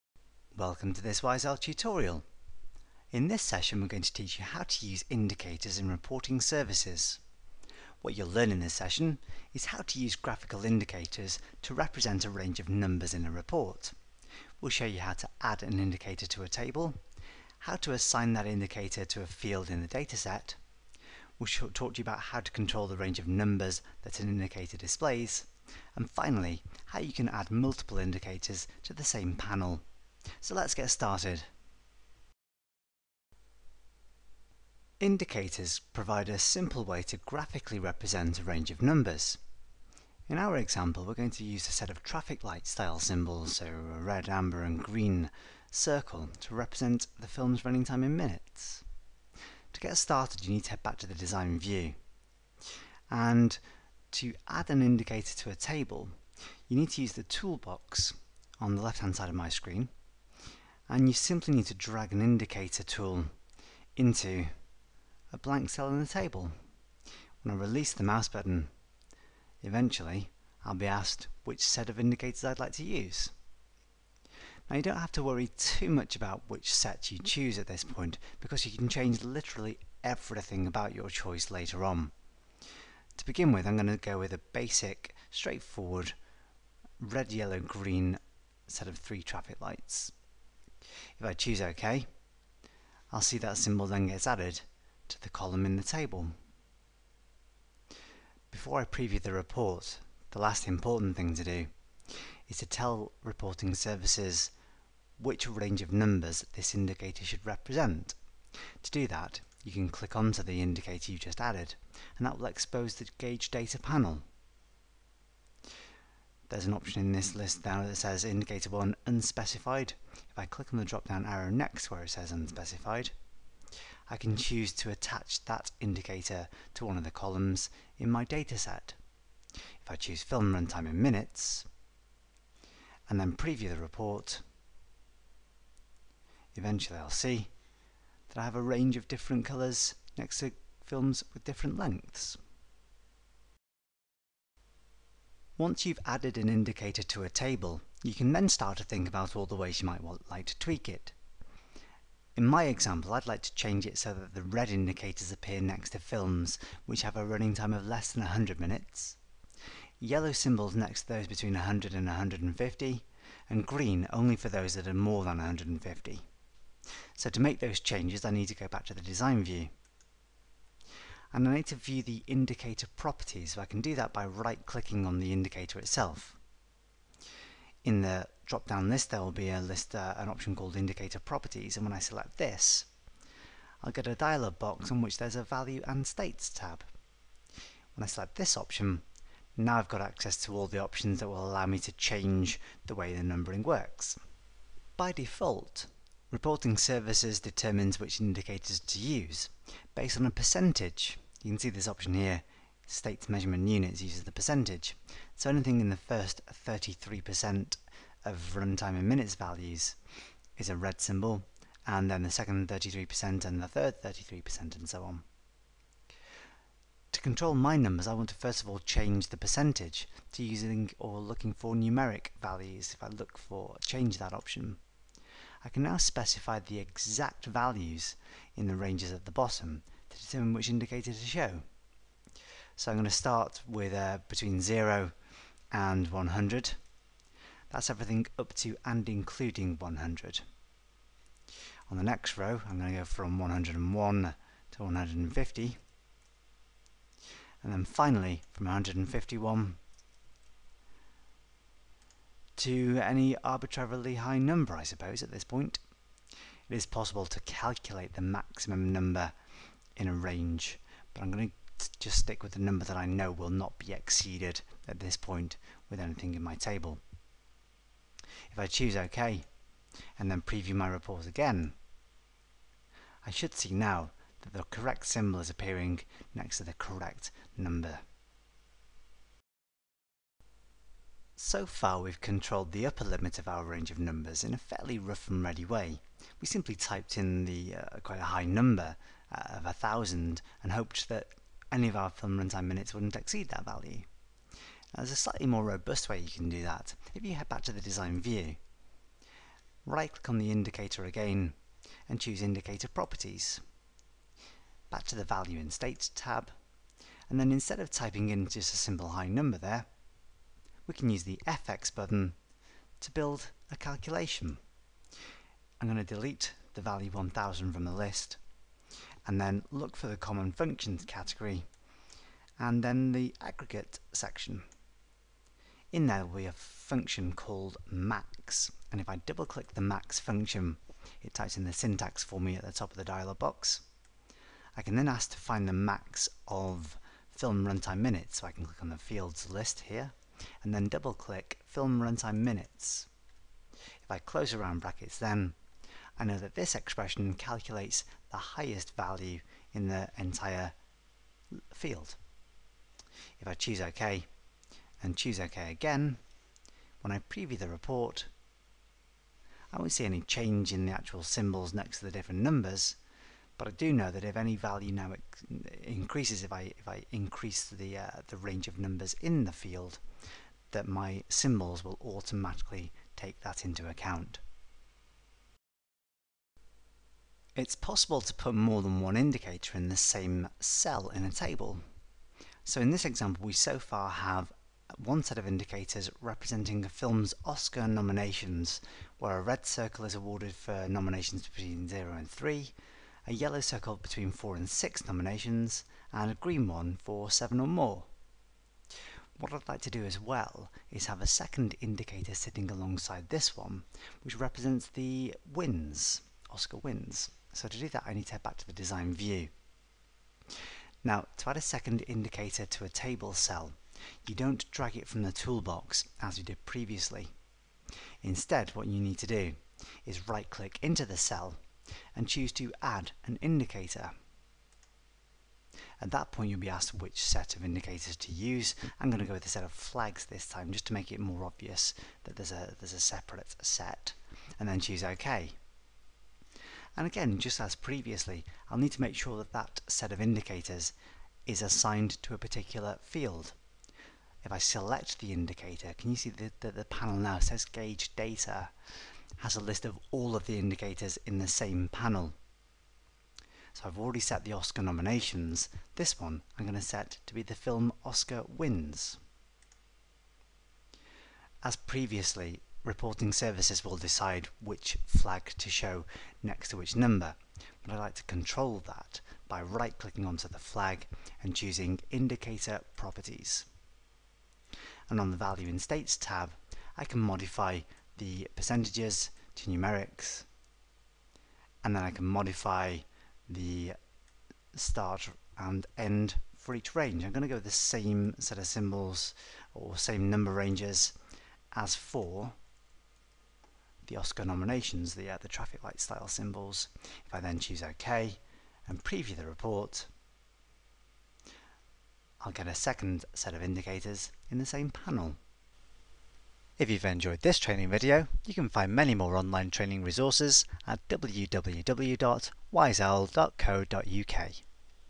Reporting Services (SSRS) Part 9 - Indicators
By Andrew Gould

- Bored of looking a list of numbers in Reporting Services reports?  This video teaches you how to visualise a range of numbers using graphical indicators.  You'll learn how to add indicators to a table, how to link indicators to fields in a dataset and how to control exactly which values your indicators represent.

You can download the script to create the Movies database used in this video at the following link: